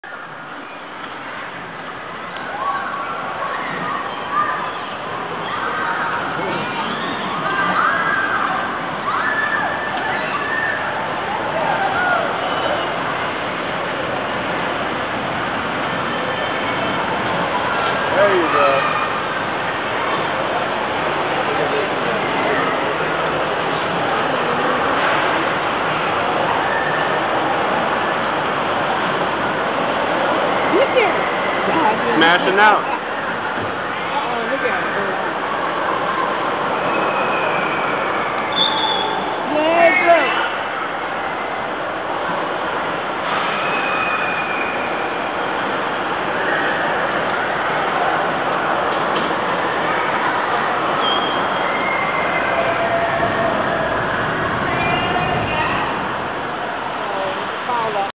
Canyon Cars
My daughters driving the canyon cars at the Adventuredome circus circus hotel and casino las vegas summer 2012